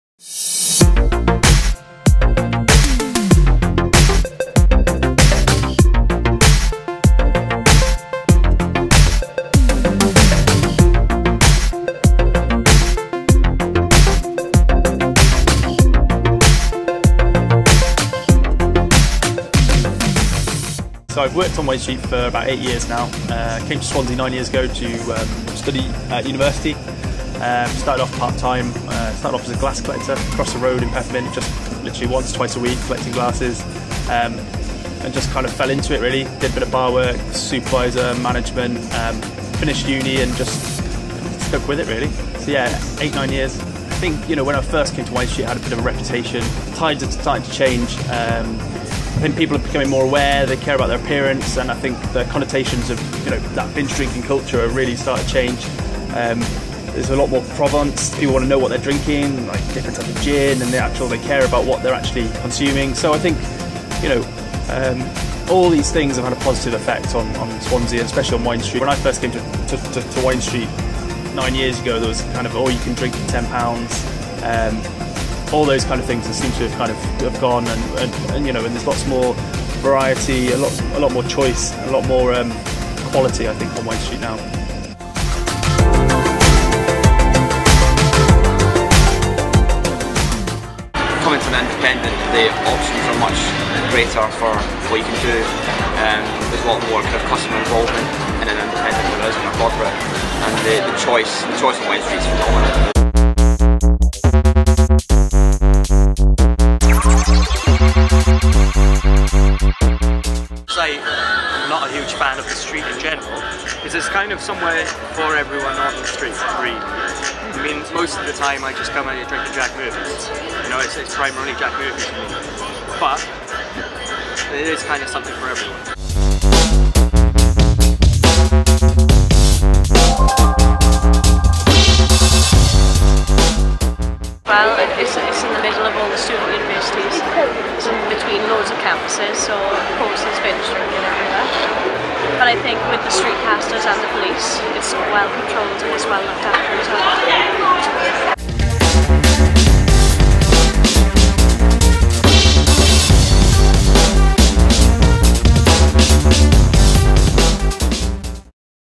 The people and places who make Swansea's Wind Street party central
Ever-evolving Wind Street has its reputations good and bad,  Megan Griffiths went to meet the people who make it tick.